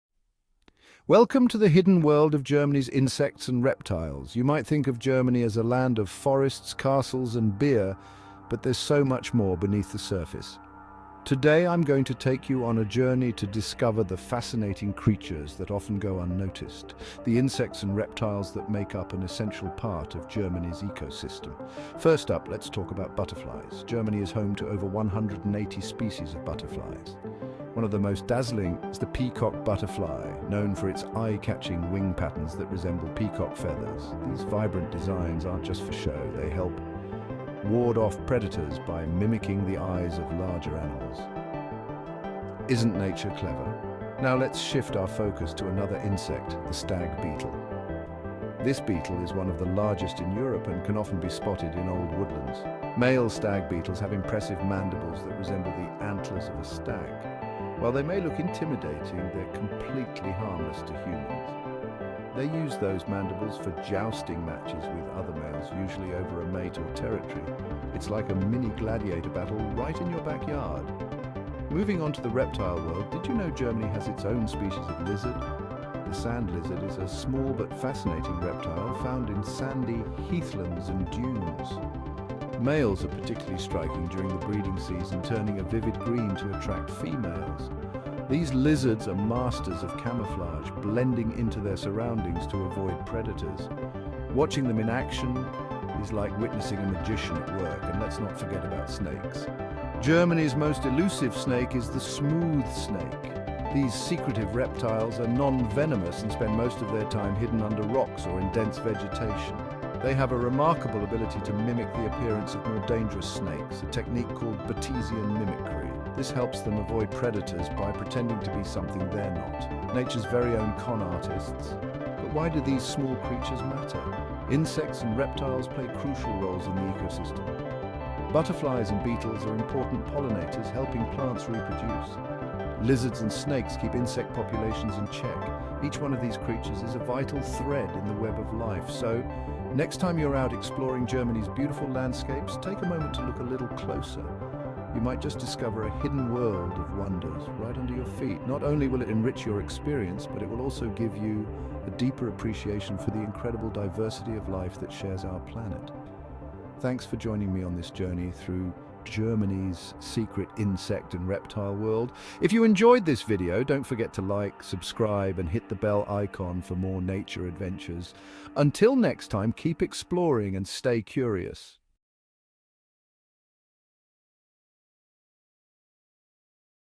Insect and Reptile Wonders: Germany’s Hidden Wildlife
Get up close with Germany’s fascinating insects and reptiles, the often overlooked yet vital components of its ecosystems! From the vibrant, fluttering wings of dazzling butterflies to the stealthy movements of elusive snakes, these creatures may be small, but they play essential roles in maintaining ecological balance. In this exploration, we’ll dive into the unique adaptations and behaviors that allow these insects and reptiles to thrive in Germany’s diverse habitats, from dense forests to sunlit meadows. Learn about the crucial services they provide, such as pollination, pest control, and serving as prey for larger animals. Through captivating footage and insightful commentary, this journey offers a new perspective on the incredible biodiversity found in these tiny but important creatures. Join us in celebrating and understanding the essential roles that Germany’s vibrant insects and reptiles play in sustaining a healthy environment.